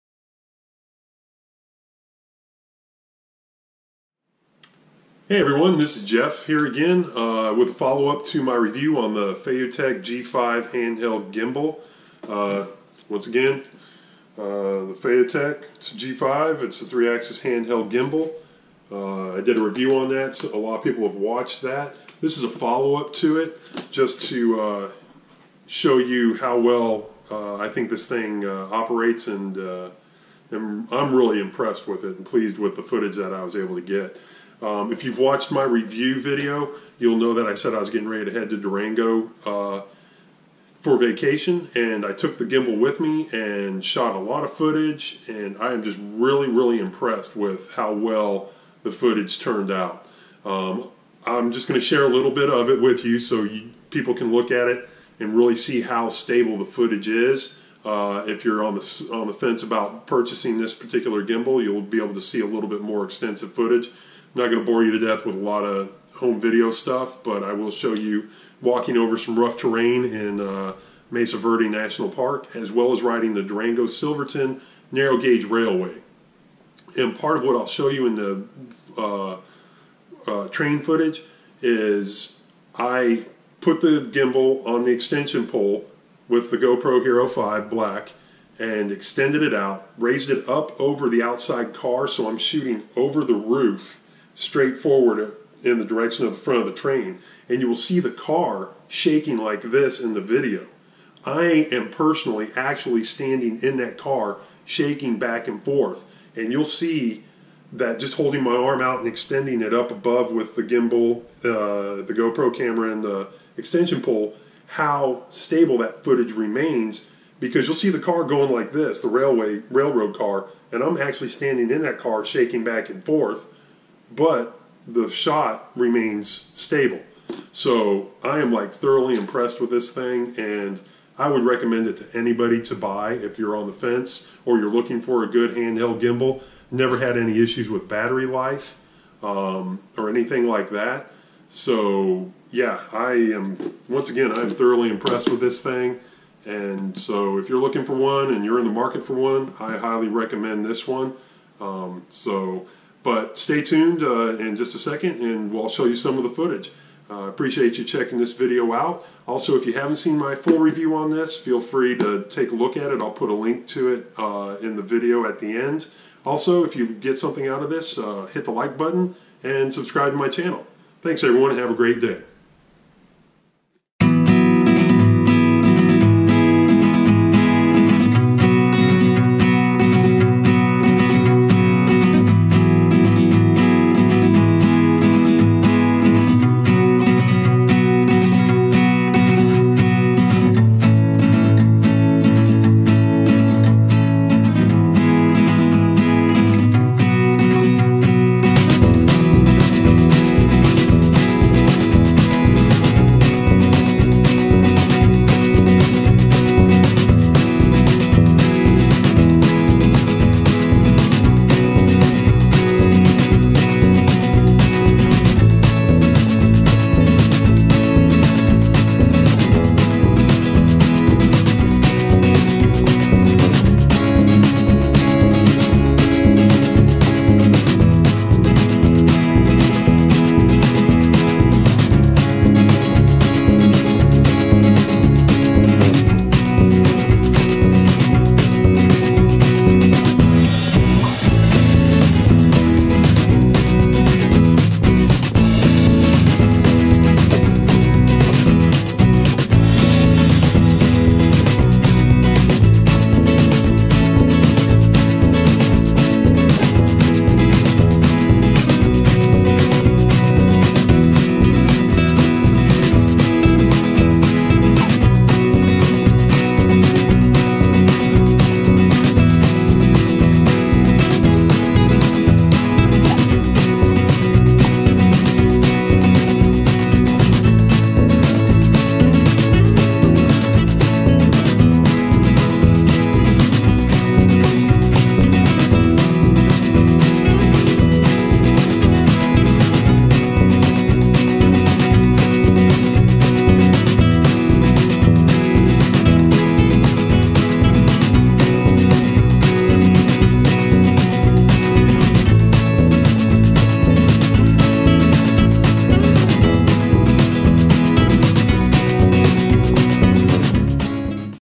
Footage Shot Using The Feiyu Tech G5 Gimbal and GoPro Hero 5 Black
This is a follow up to my review on the Feiyu Tech G5 Handheld Gimbal.  This is footage that I shot with my GoPro Hero 5 Black using the Feiyu Tech G5.  The smoothness of the footage is remarkable!  I am thoroughly IMPRESSED with this piece of equipment!  I can't wait to use it again and again! I am not being paid to say this.  This is my honest opinion.   THE LINK TO MY FULL REVIEW OF THIS GIMBAL IS LOCATED AT THE END OF THIS VIDEO